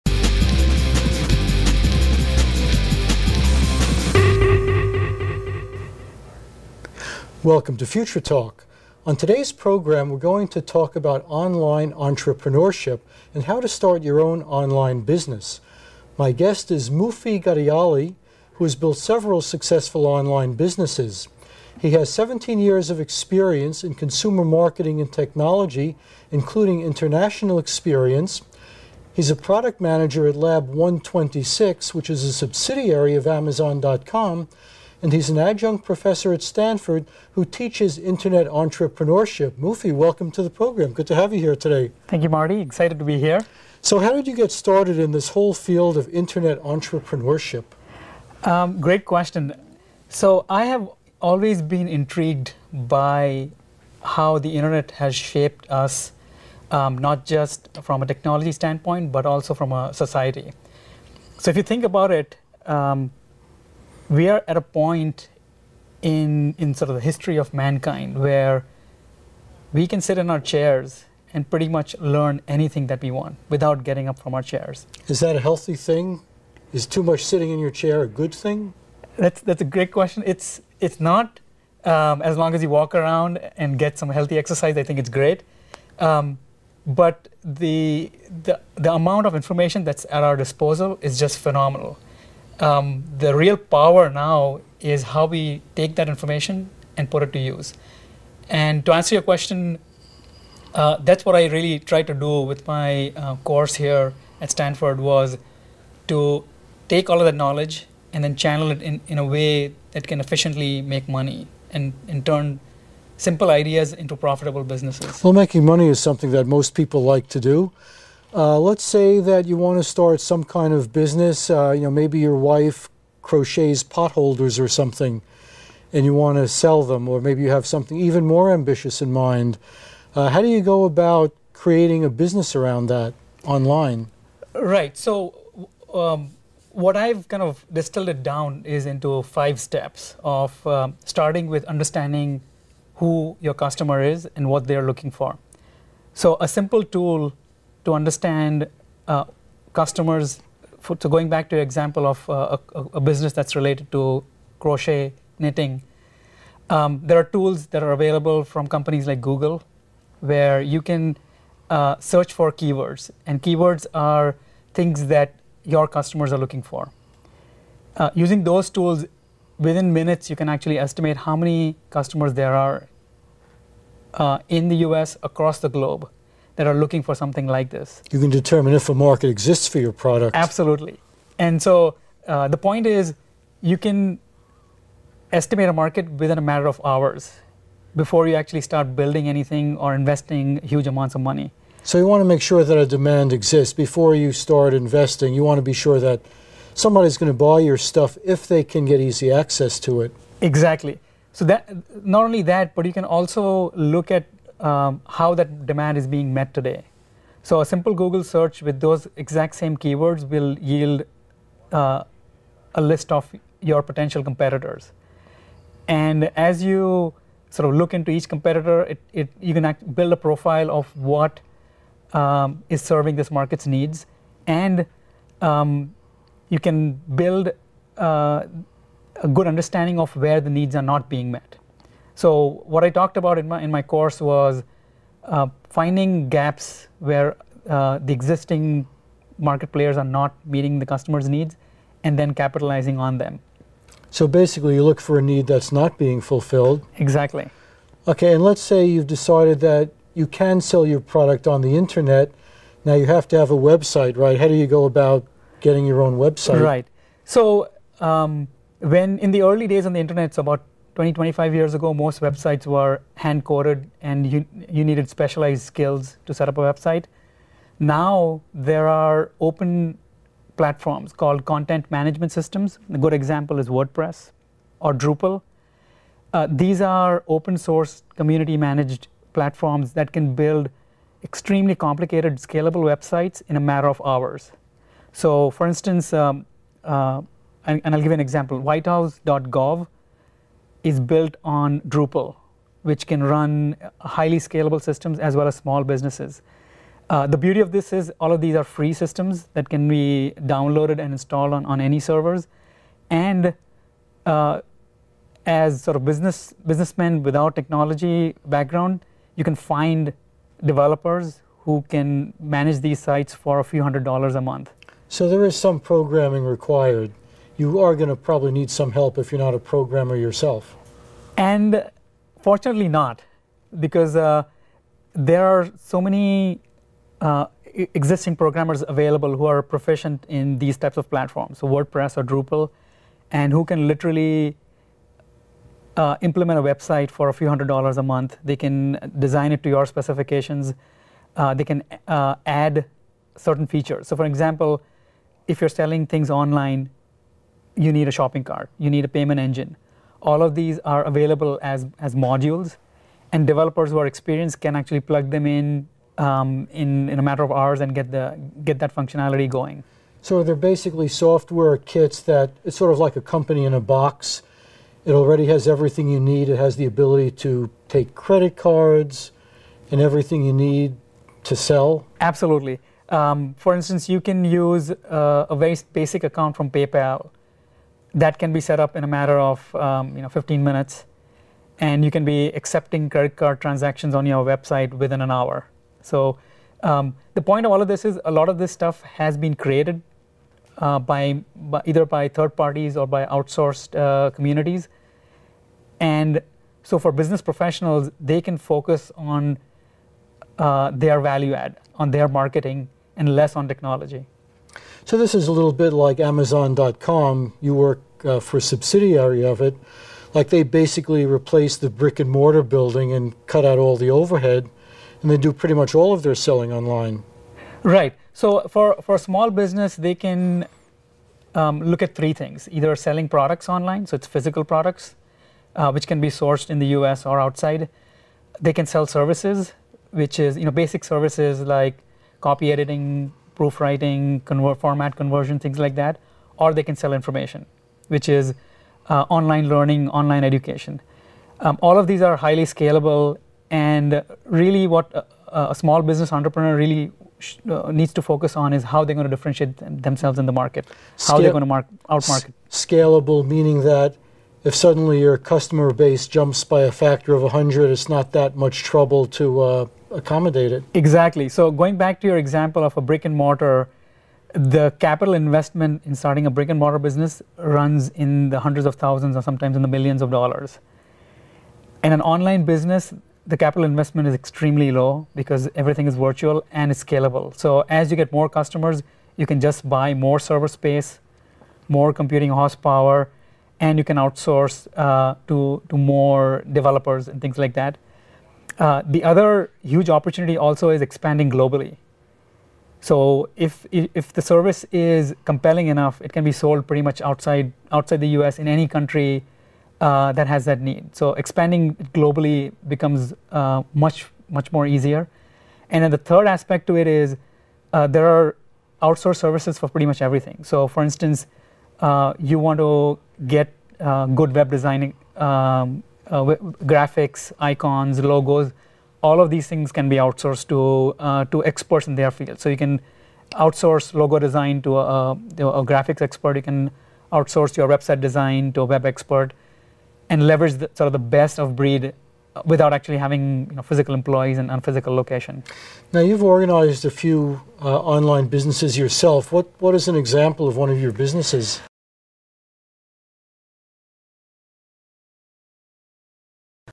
Future Talk 31-1, Starting an Online Business
An interview with Muffi Ghadiali, Adjunct Professor at Stanford University who teaches courses on Internet entrepreneurship and who has started several online companies himself, on the benefits of starting an online business.